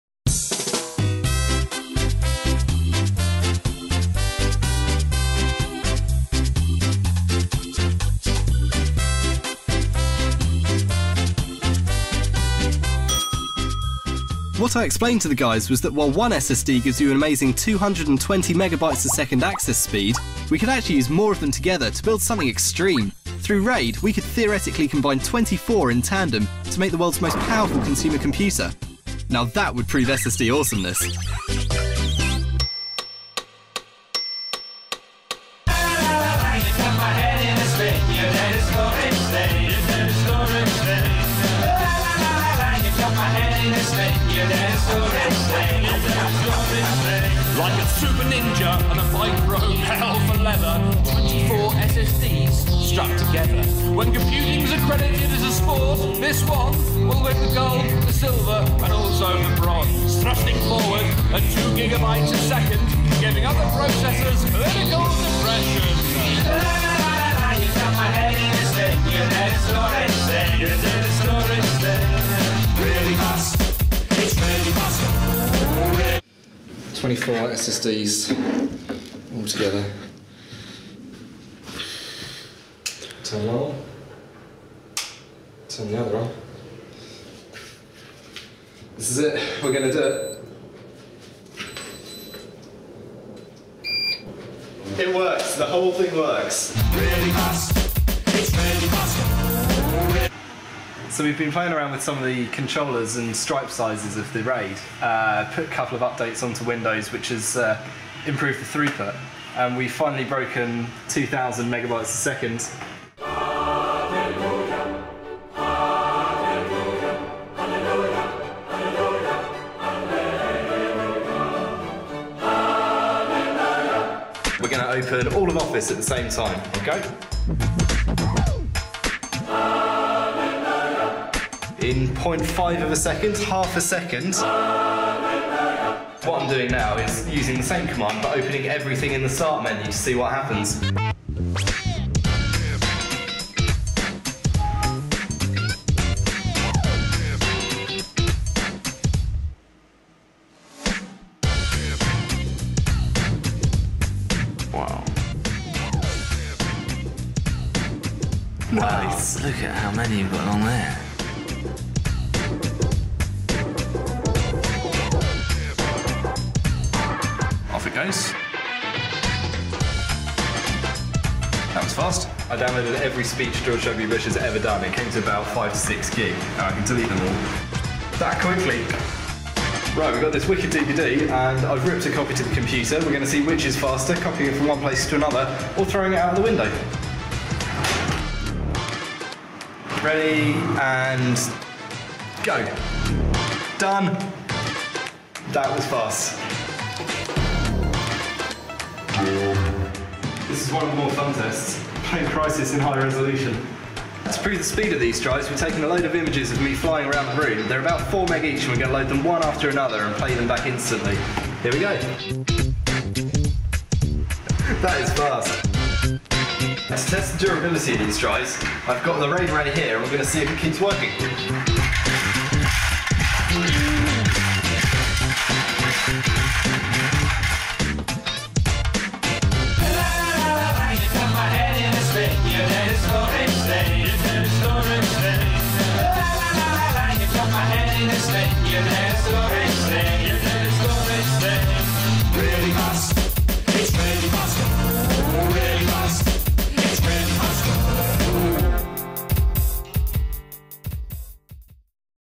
Samsung SSD Awesomeness
We took 24 256GB Samsung MLC SSD's and put them in RAID to make this awesome computer! See how we did it, and what the results were!